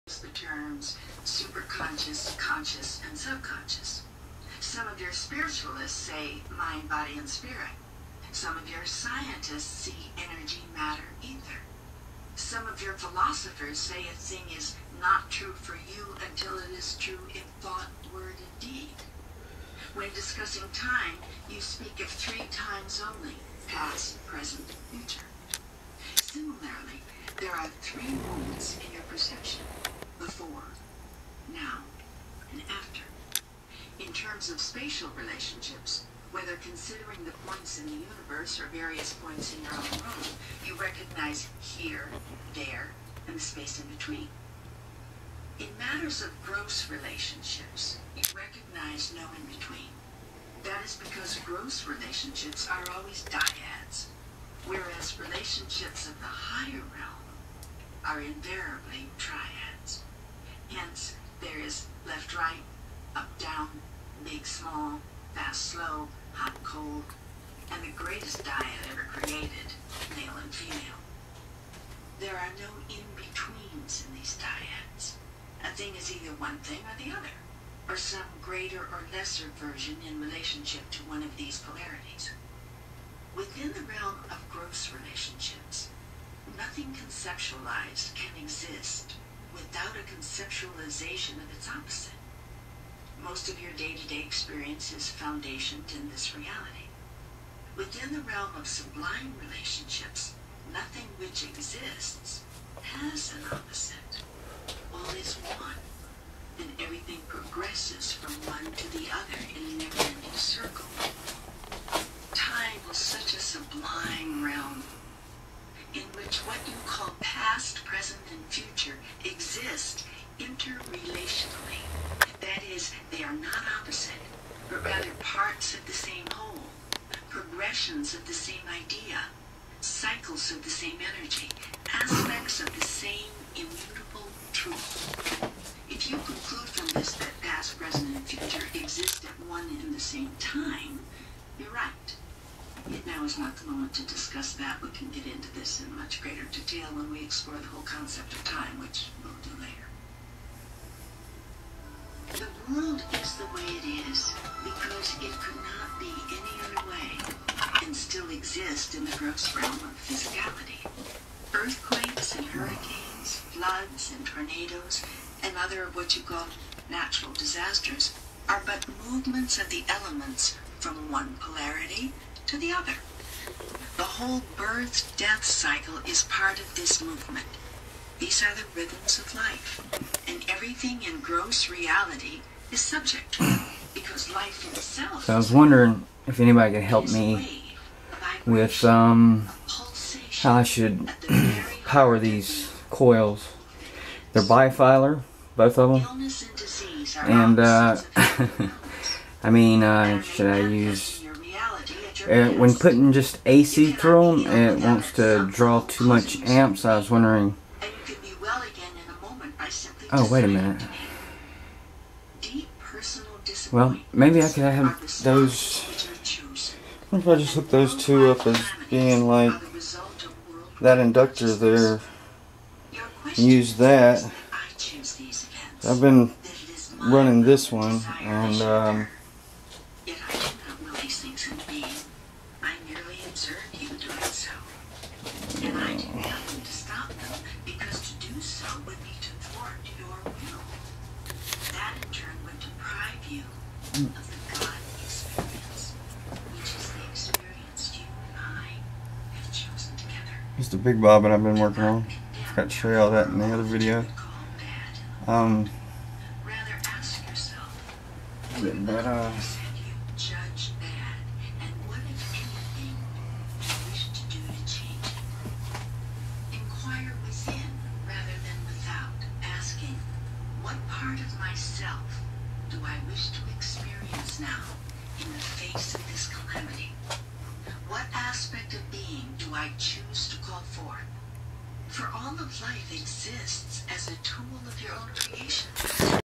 Primer field coils.how should I power?
Scope shot, water cell wave form...I was looking for my field detector at the end got side tracked...lol